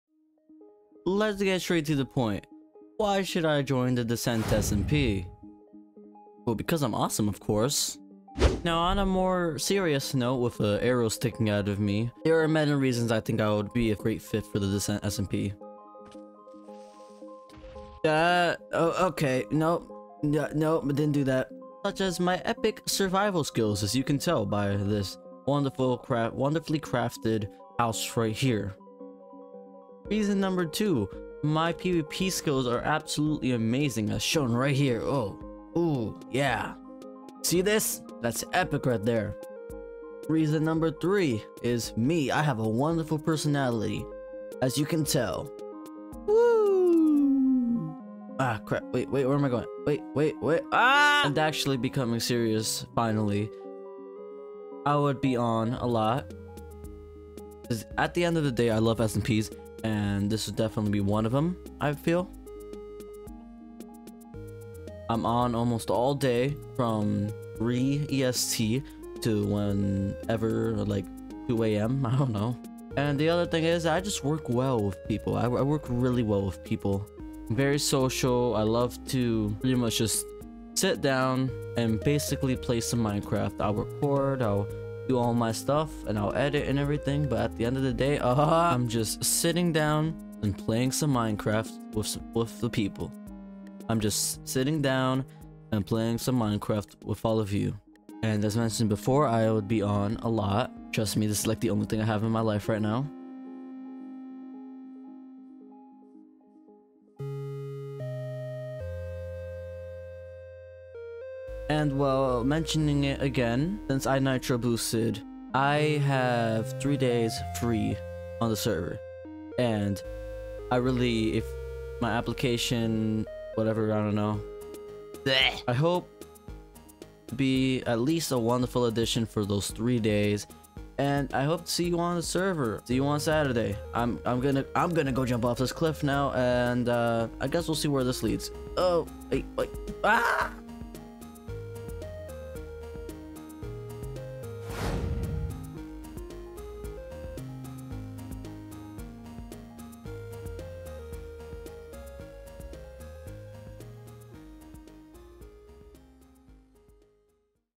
Spy Live's Application to the Descent SMP! [accepted cuz i'm cool]
Hello and welcome to my official application to the Descent SMP.
I'm not going to make the description detailed as this video is unlisted.